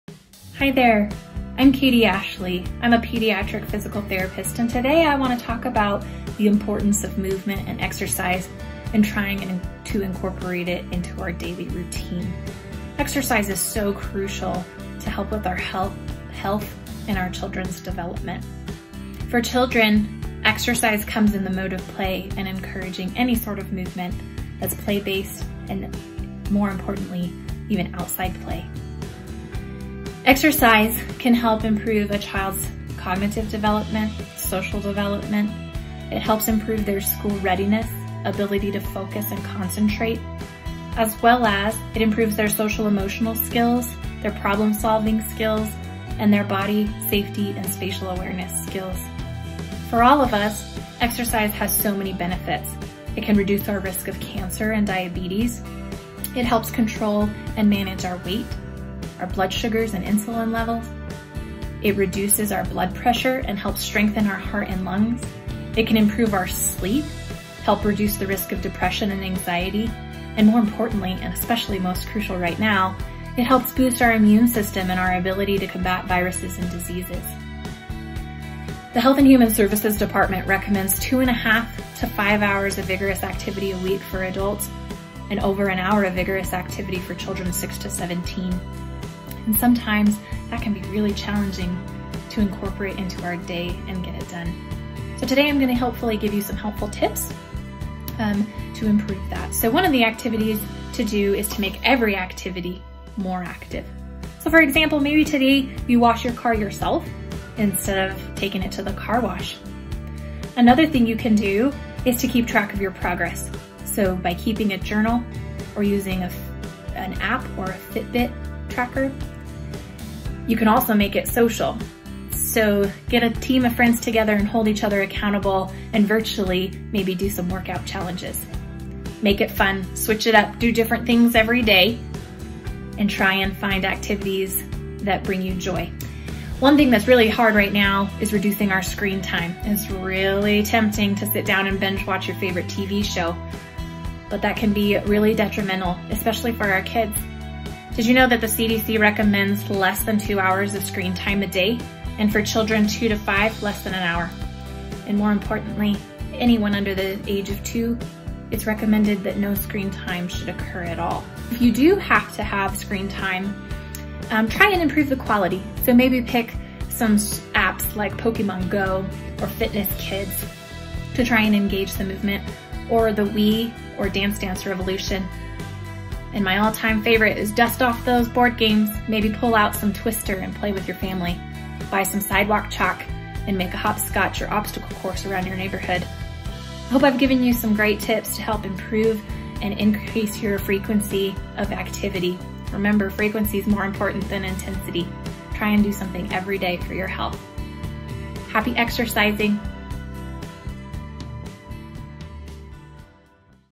Exercise Is Essential
We all know that exercise is essential for good health. But, did you know exercise also boosts our immune system, helping us fight viruses? Katie Ashley, a Pediatric Physical Therapist at San Juan Regional Medical Center, shares great tips and advice on how to incorporate more exercise into our busy lives.

Start exercising more by getting out on the beautiful trails in area